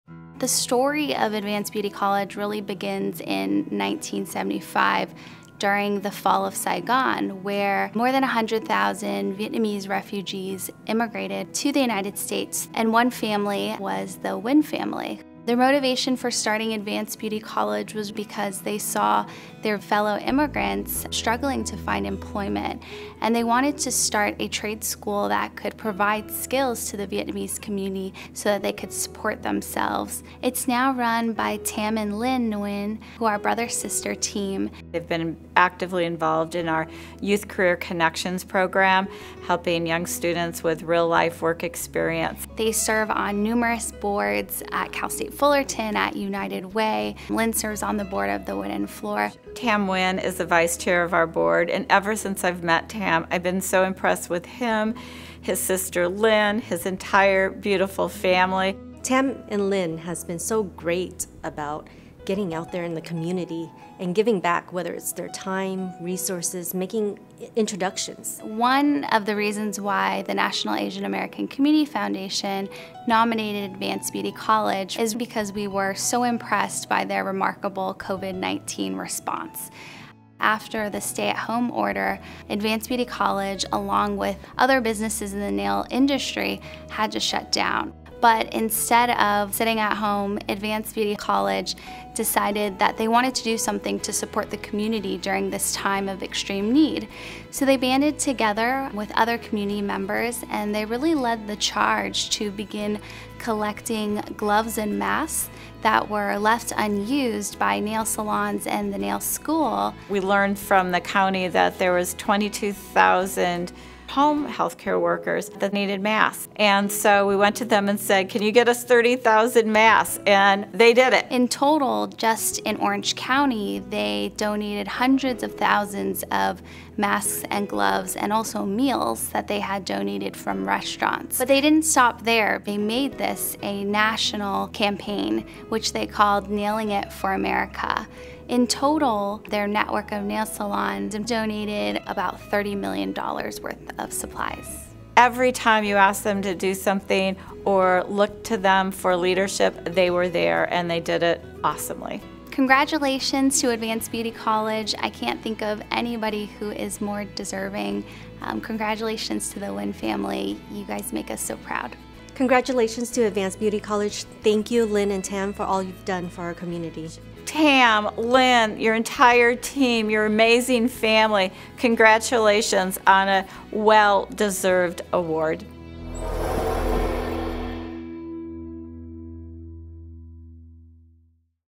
2020 National Philanthropy Day Outstanding Corporation or Small Business: Advance Beauty College
The 35th Anniversary of National Philanthropy Day, which began here in Orange County, was celebrated on November 19th, 2020. The event, which is usually held in front of a live audience, was produced virtually in its entirety this year. The event honors the amazing individuals and organizations that lead Orange County's thriving philanthropic community, and exceeded its 2020 goal in raising funds for nonprofit education and scholarships in Orange County.

Advance Beauty College ranks among the top beauty colleges in the United States with campuses in Garden Grove and Laguna Hills, Calif. More than 40,000 graduates have gone through the college’s innovative program in its 33-year history with award-winning training that includes educators who speak English, Farsi, Spanish and Vietnamese. The vast majority of graduates are proudly employed in the beauty-care industry with many becoming business owners and industry leaders themselves. Advance Beauty College was teaming with 50 faculty members and 400 students receiving hands-on training as manicurists, estheticians, hairstylists and barbers. But, then the coronavirus (COVID-19) pandemic hit California. Overnight, the college executed distance learning and became a “hub of love,” by spearheading a national campaign — Nailing It For America — to deliver desperately needed personal protective equipment (PPEs) to healthcare workers fighting COVID-19. With its core values of “Inspire, Educate and Give Back,” college volunteers coordinated delivery of 1.2 million PPEs and 55,000 restaurant meals worth $30 million to healthcare workers and other front-line heroes to more than 150 locations throughout the U.S.